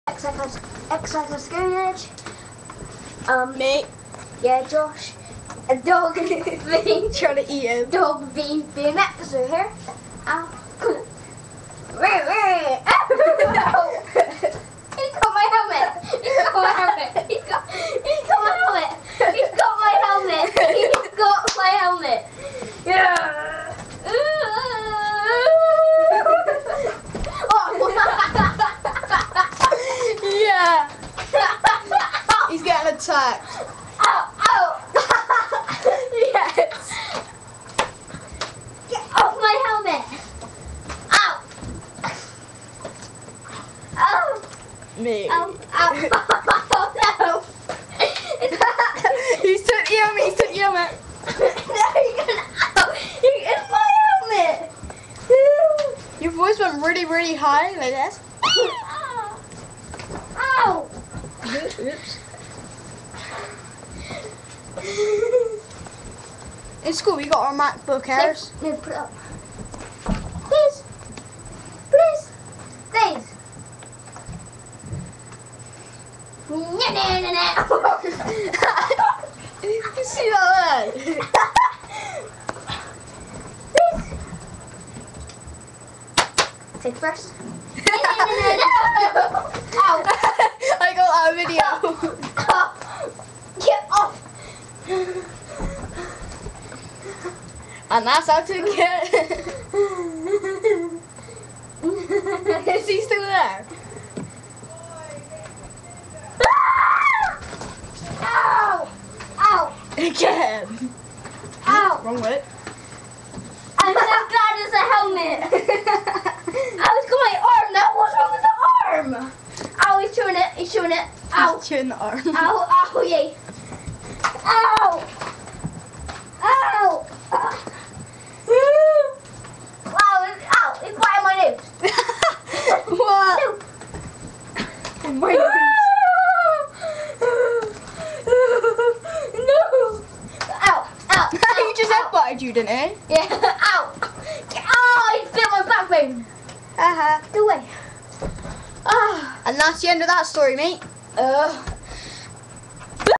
DOG VS BMX RIDER KINDA
IM THE BMXER (JOEY) AND BLAZE IS THE DOG JOSH IS THE COMMENTATOR